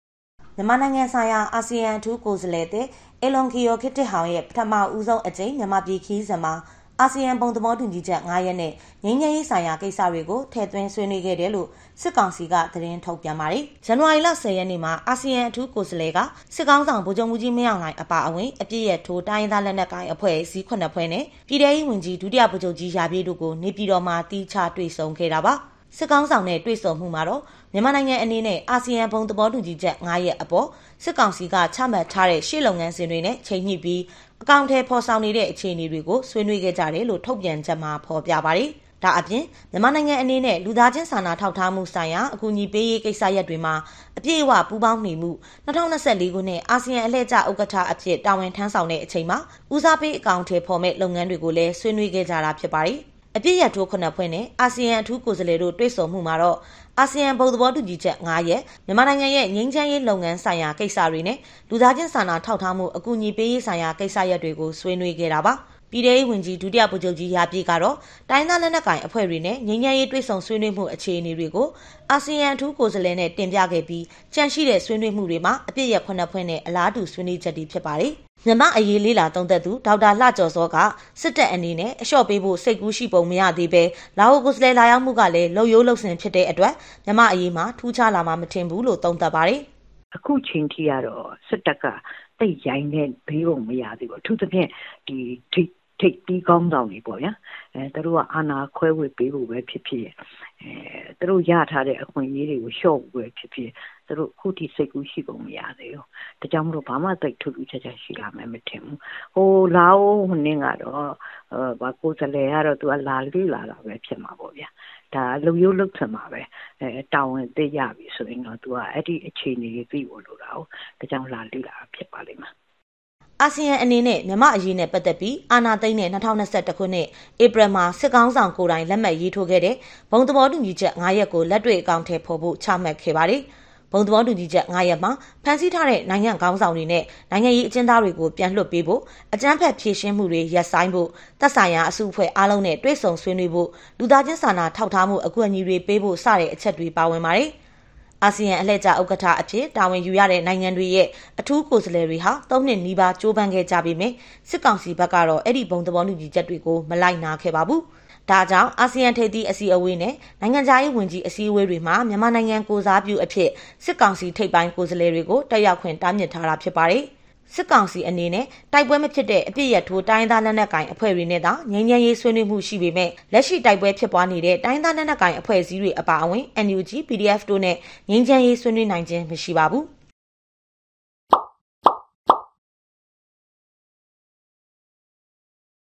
TLT Tv||- မြန်မာ့အရေး အာဆီယံကြိုးပမ်းမှု စစ်ကောင်စီကြောင့် အလုပ်မဖြစ်နိုင်ဘူးလို့ သုံးသပ်သူတွေ ပြော
မြန်မာနိုင်ငံဆိုင်ရာ အာဆီယံအထူးကိုယ်စားလှယ်သစ် အေလွန်ကီရို ကစ်တစ်ဟောင်ရဲ့ ပထမဆုံးအကြိမ် မြန်မာပြည်ခရီးစဉ်မှာ အာဆီယံဘုံသဘောတူညီချက် ၅ ရပ်နဲ့ ငြိမ်းချမ်း‌ရေးဆိုင်ရာ ကိစ္စတွေကို ထည့်သွင်းဆွေးနွေးခဲ့တယ်လို့ စစ်ကောင်စီက သတင်းထုတ်ပြန်ပါတယ်။